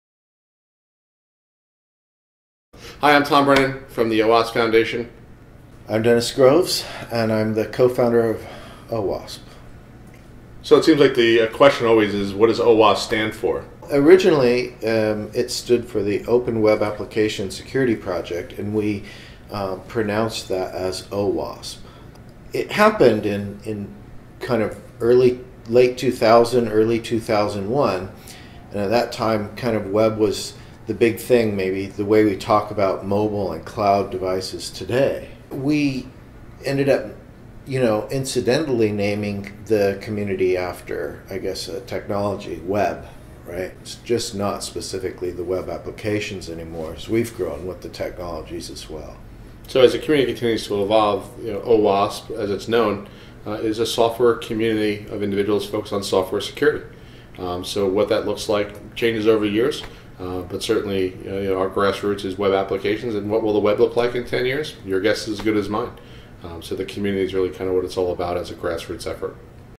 About the name of OWASP by Dennis Groves and Tom Brennan
Dennis Groves (OWASP Co-Founder) and Tom Brennan (OWASP Global Board member) talk about the meaning of OWASP and what it stands for.